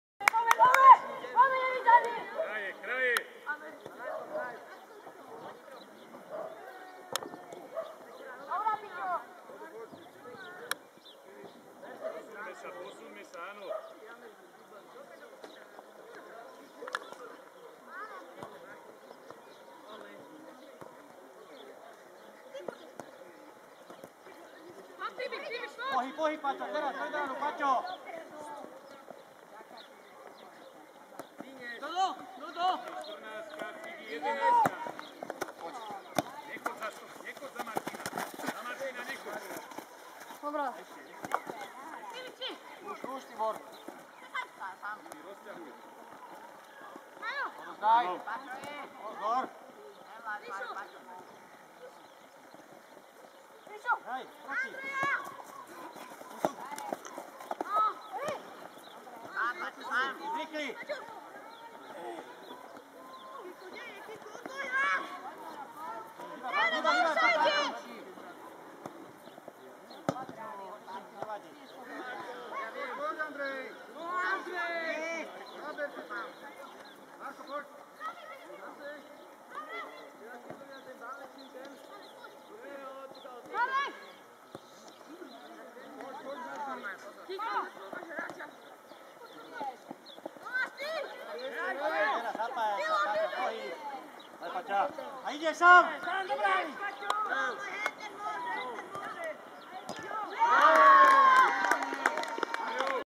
Gól Vrábľov na 2:5 (Nevidzany U15 - Vráble U13 6:2)
prípravný zápas, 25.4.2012 o 16:00, Nevidzany

NEVIDZANY U15 - PFA VION VRÁBLE U13 6:2 (2:1)